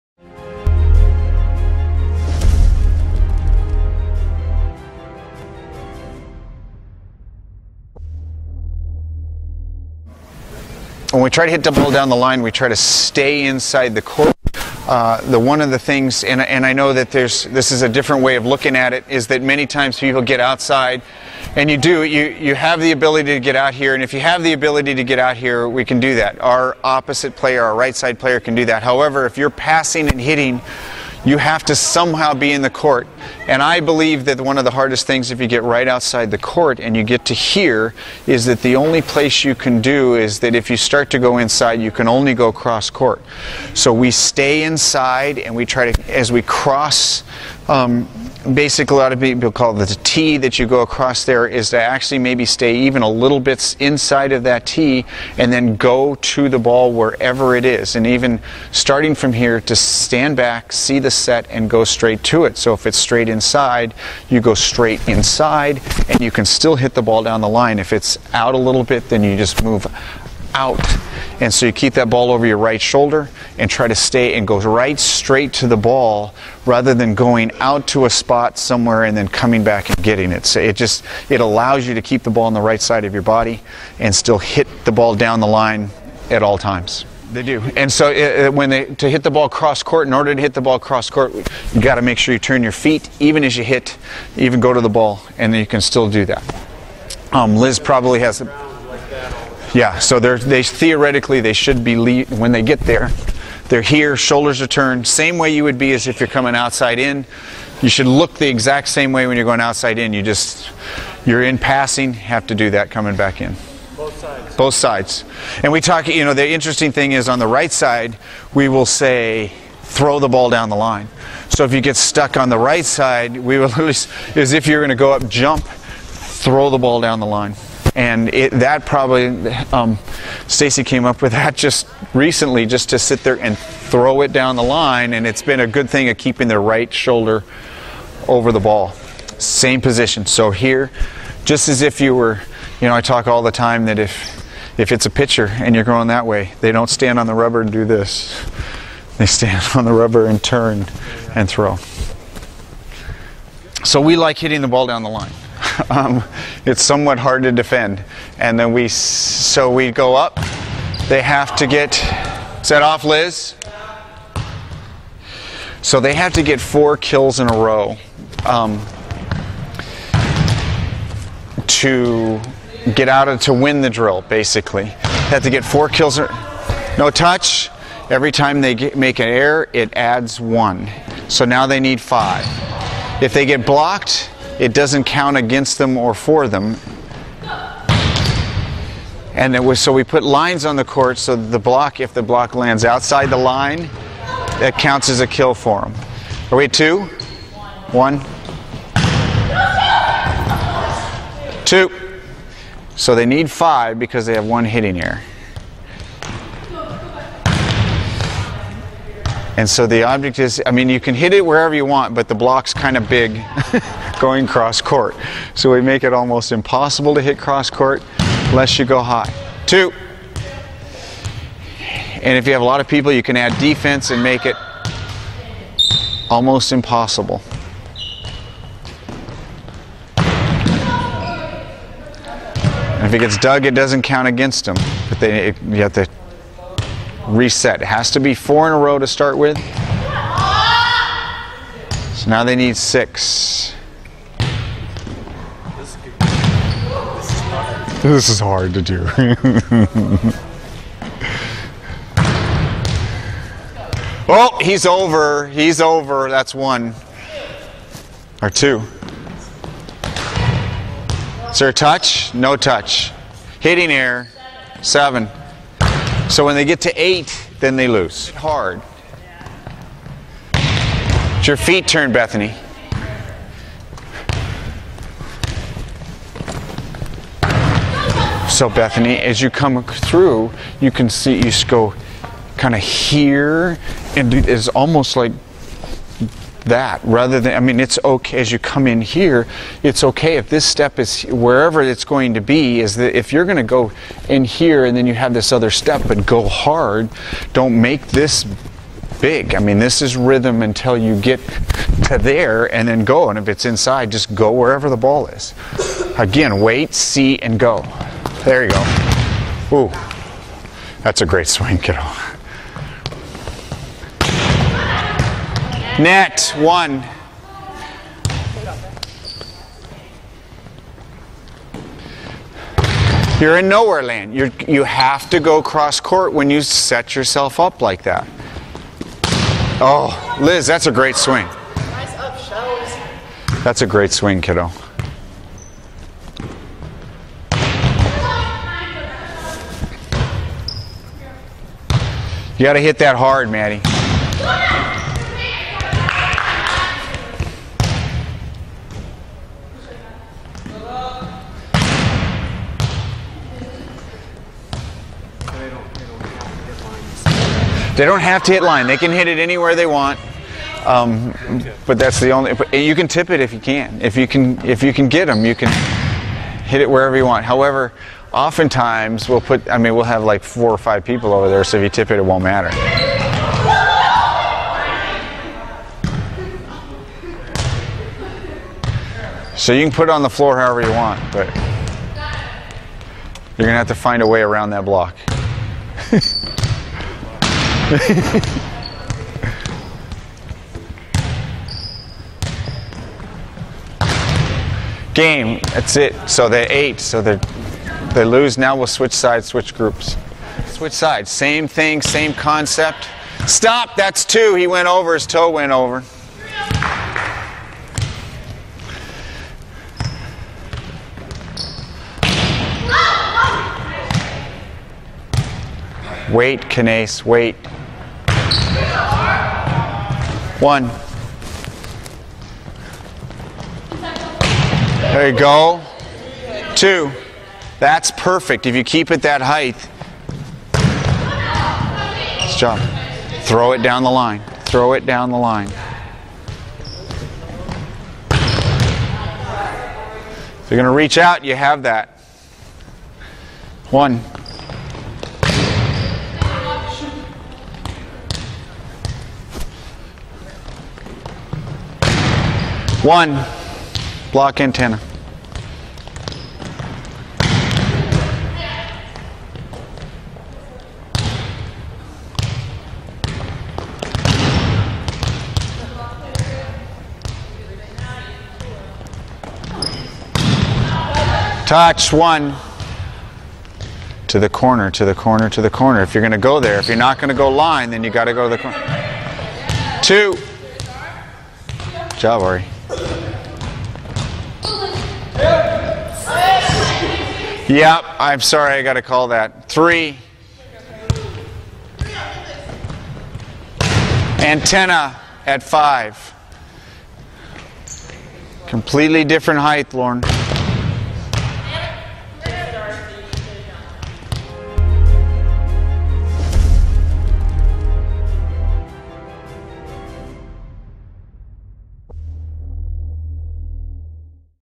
Oregon Ducks Volleyball Line Shot Drill
In this drill, Oregon University shows a drill that they use in order to work on their pin hitters hitting the line shot better on both the left and right sides of the court.